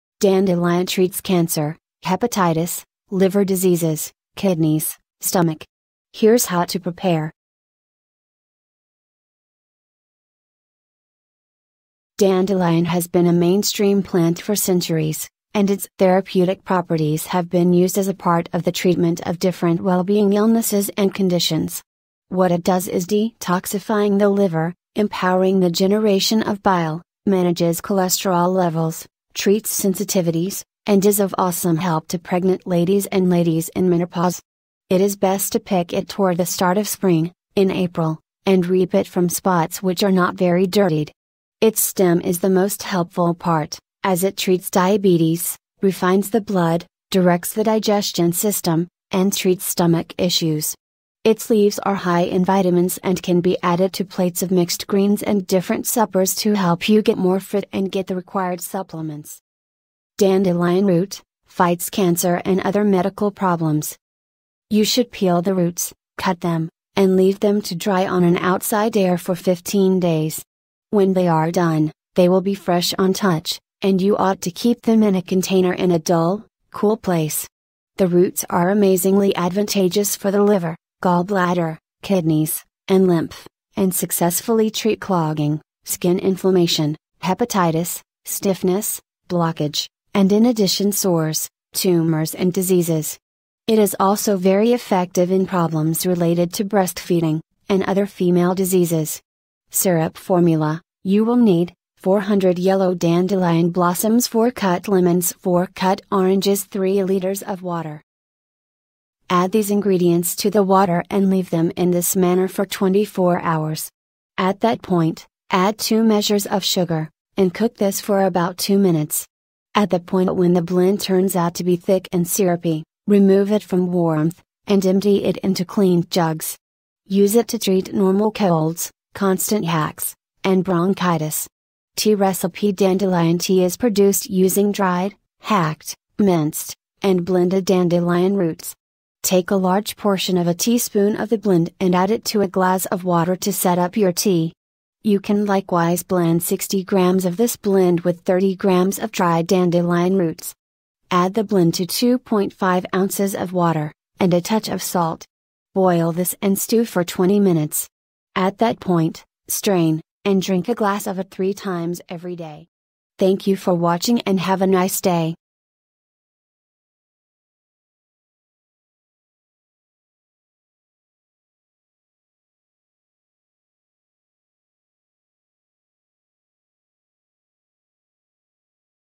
Dandelion Treats Cancer, Hepatitis, Liver Diseases, Kidneys, Stomach… Here’s How To Prepare!
Dandelion has been a popular plant for millennia, and its medicinal properties have been used in the treatment of various health ailments and conditions.

 Kidneys
Liver Diseases
Hepatitis
hcv
hepatitis b treatment